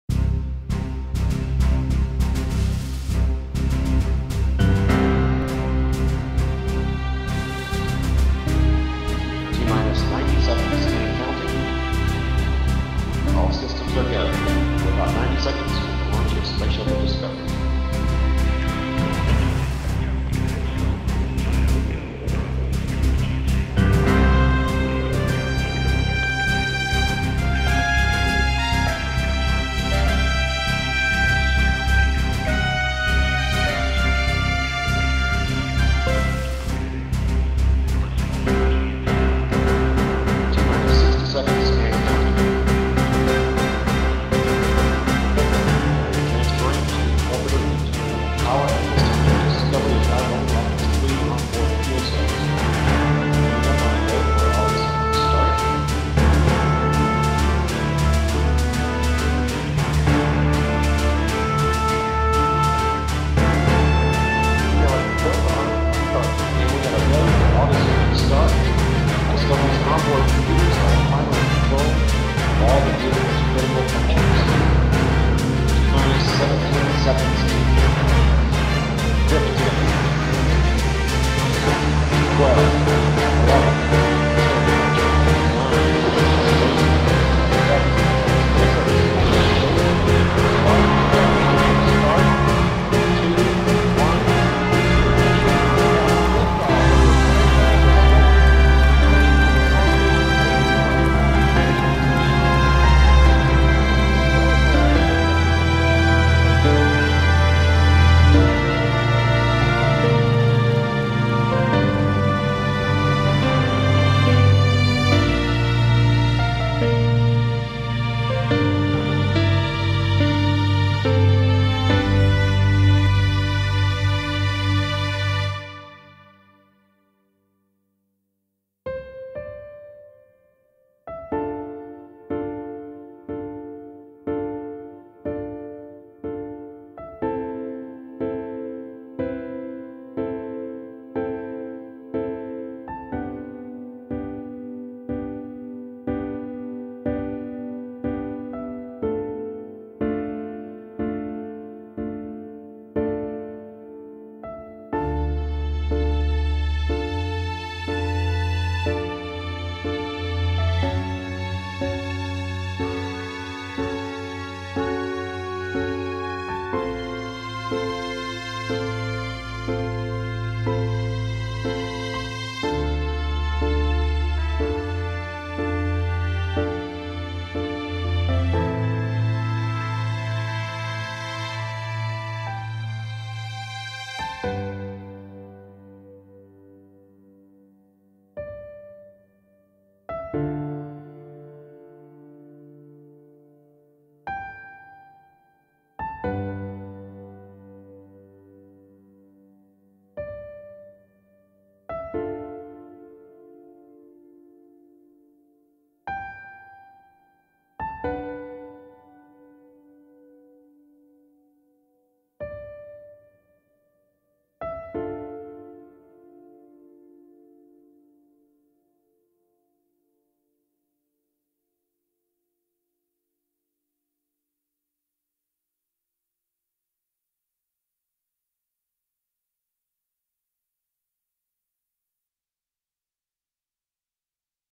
Film Scoring Exercise - Nasa Launch
Composed, Secuenced and Orquestated by Carlos Lavín
Mixing-Felipe Suil
Mastering - Guillermo Andres Ortega Contreras